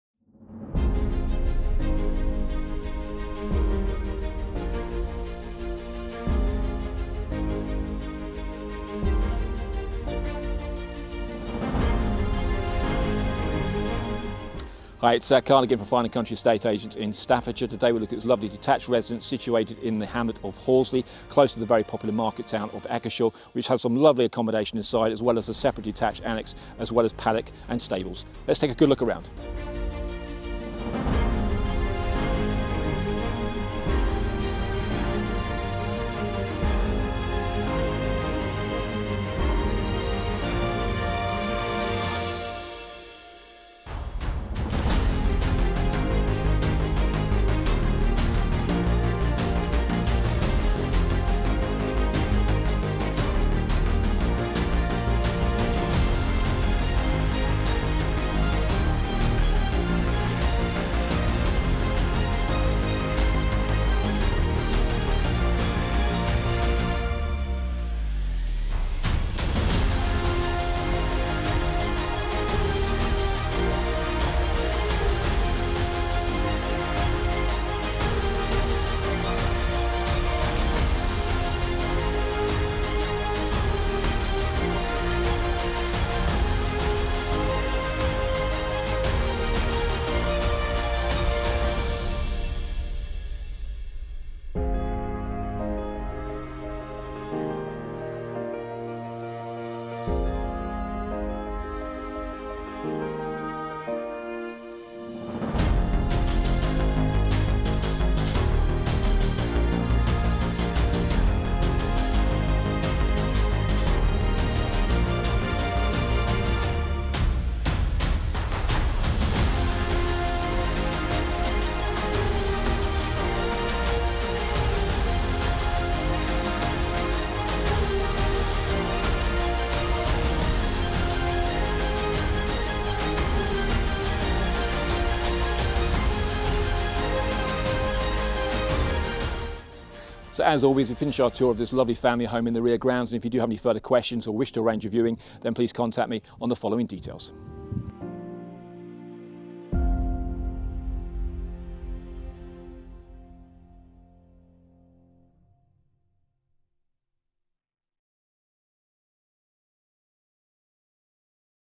Horsley, Eccleshall | Fine & Country Staffordshire | Karl Rusk
Dairy House is a lovely detached equestrian property sitting in just under 3 acres of land including paddocks, stables and private gardens in the hamlet of Horsley and close to the popular and charming market town of Eccleshall.

This family home includes 4 bedrooms 2 bathrooms and 3 receptions with the addition of a sunroom. There is also a separate detached outbuilding currently used as a gym and tack room which can be converted into a fully fledged annexe.

In summary a well presented home in a quiet location.

Freehold | EPC Rating: E | Council Tax Band: G | Local Authority: Staffordshire County Council & Stafford Borough Council

Offers Over £1,250,000

Karl Rusk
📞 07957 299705

Fine & Country Staffordshire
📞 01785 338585